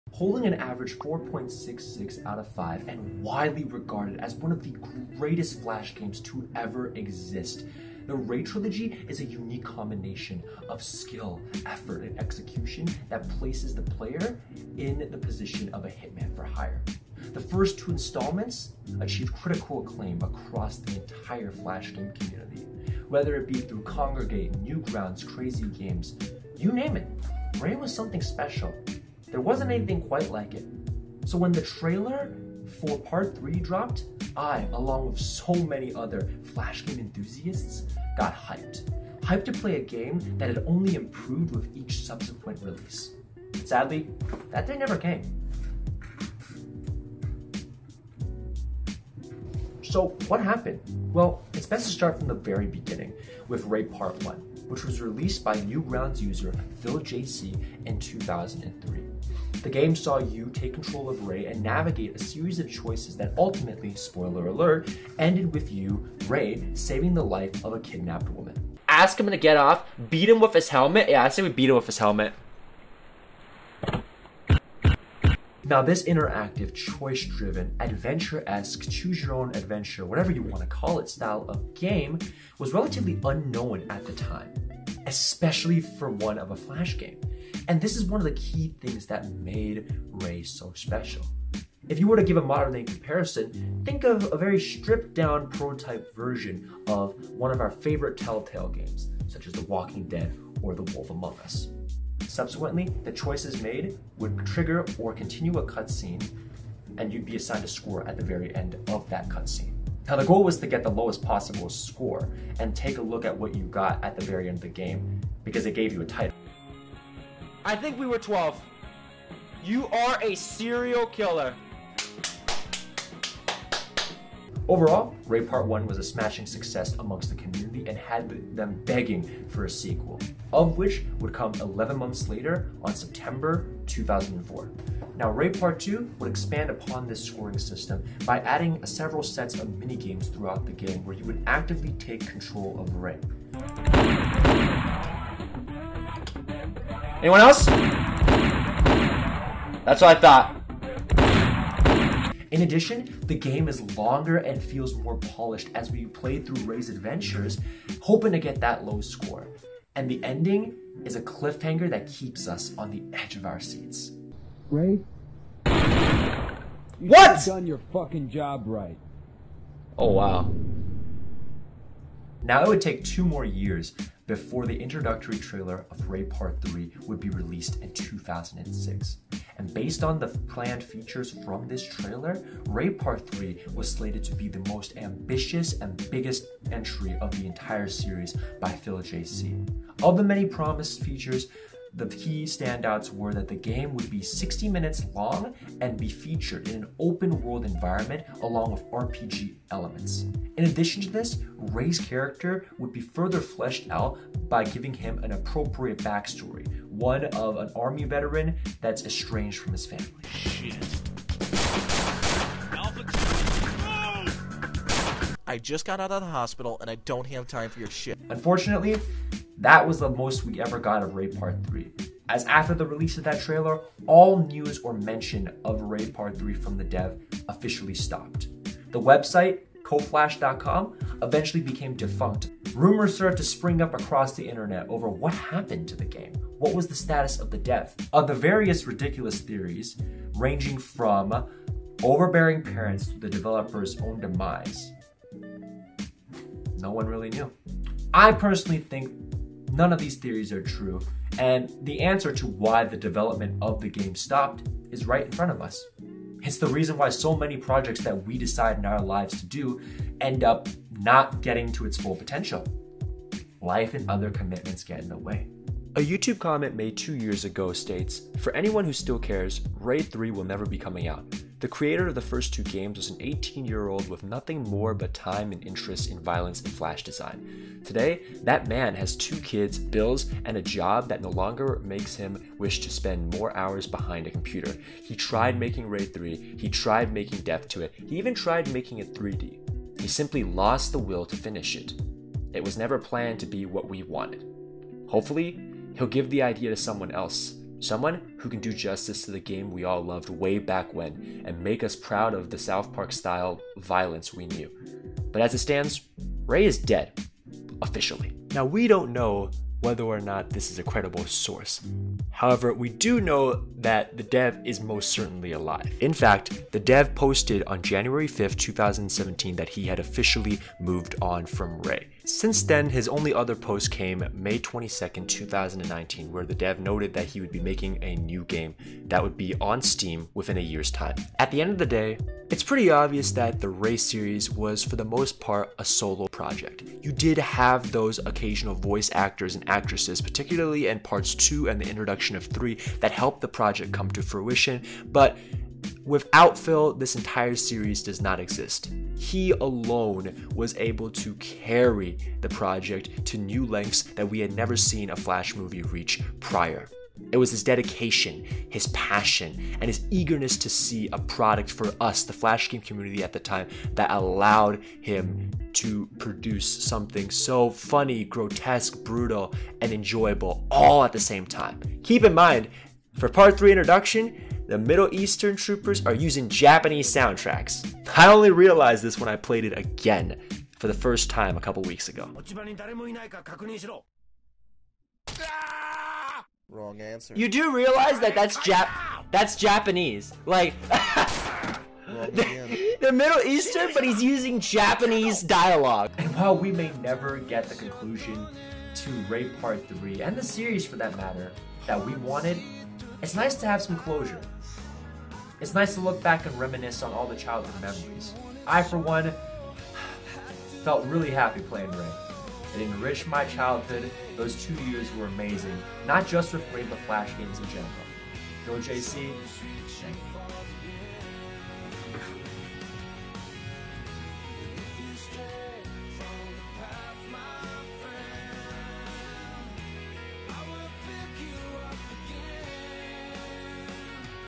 The Story of the Ray Flash Games
Today we explore one of the greatest flash games series ever made and why it remains unfinished.